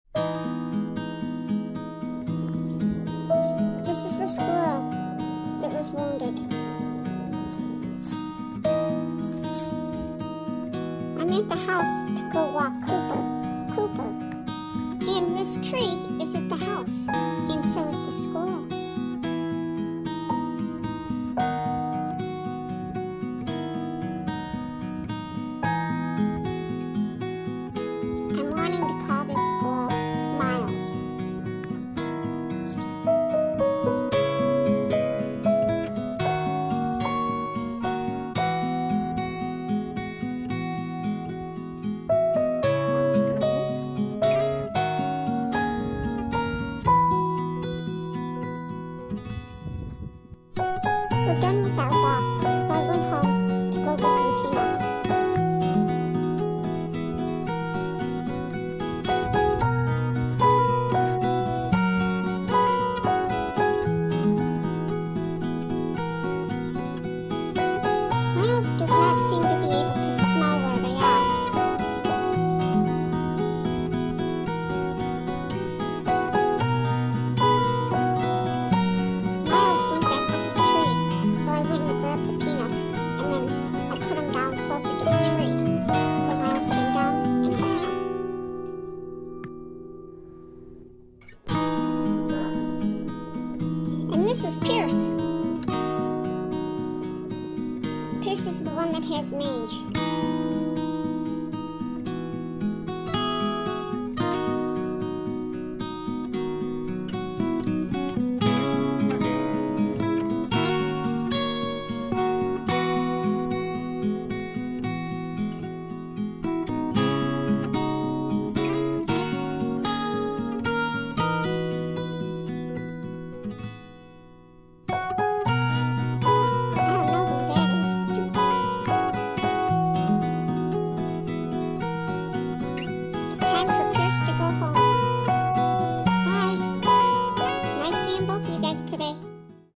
Feeding an injured squirrel and one that has Mange.
The injured squirrel was at a tree where I walk 2 dogs. I was happy Miles, name I gave the injured squirrel, was still there after our walk. And seeing Pierce, who has Mange, was an answer to my prayer!